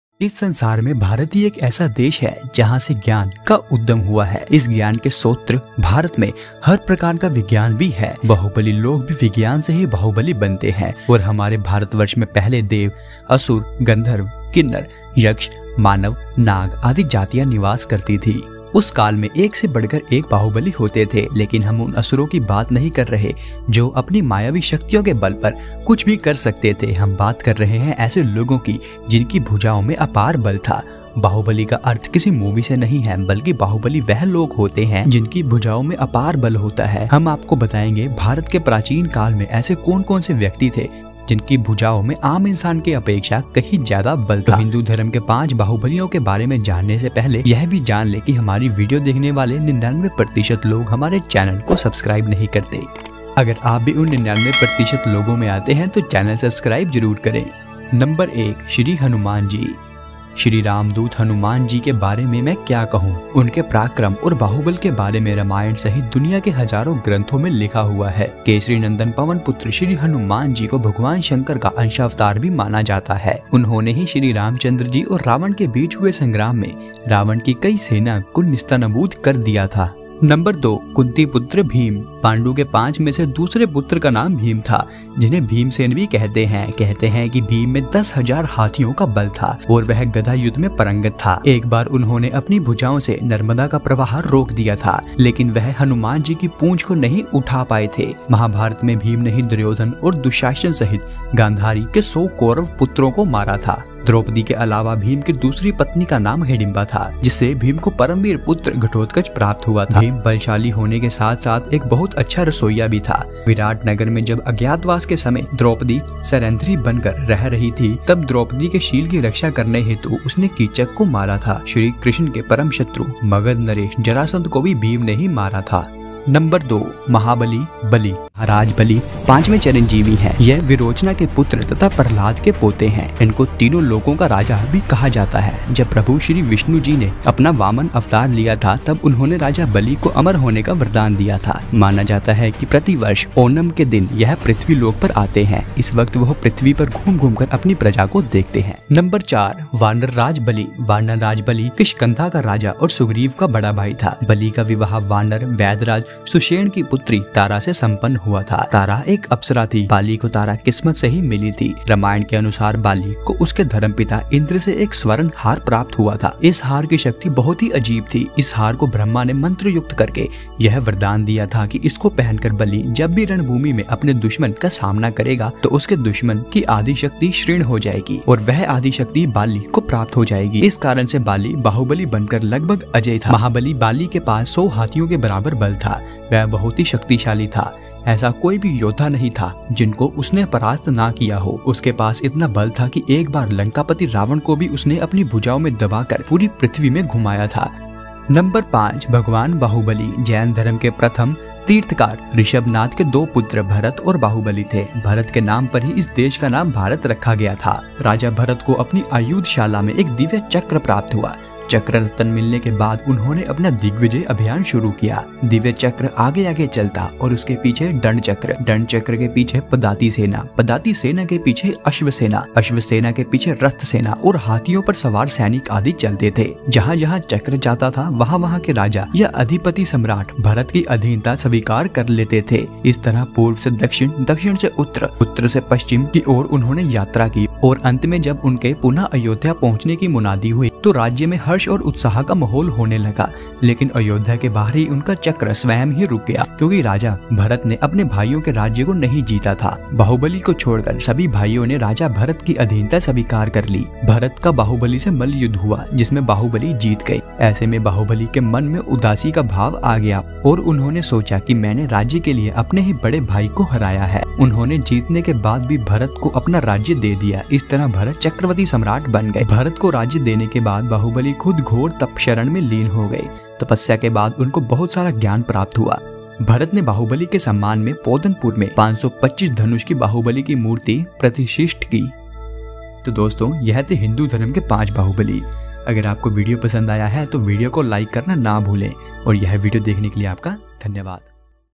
हिंदू धर्म के 5 बाहुबली | 5 Strongest Warriors of Hindu Religion | Gyan Villa
1. Hanumanji
2. Bhimsen
3. Raja Bali
4.  Vanar Raj Baali
5. Bhagwan Bahubali ji